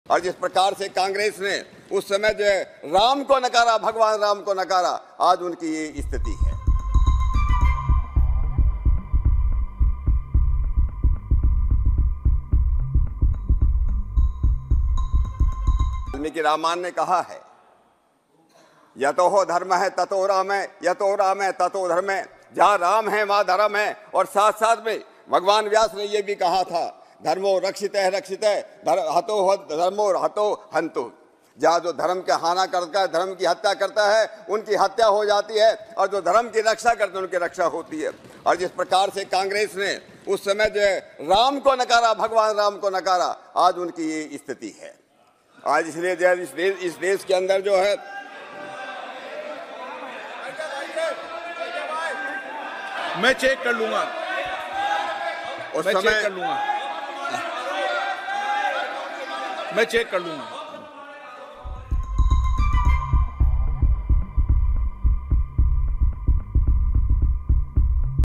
BJP MP Satya Pal's Scathing Attack On Congress, "Those Who Destroy Dharma..."
Parliament Session: While discussing Ram Temple on the last day of the 17th Parliament session, BJP MP Satya Pal lodges a scathing attack at Congress. Quoting Vyas, the MP claims that, "Those who destroy Dharma are destroyed by Dharma".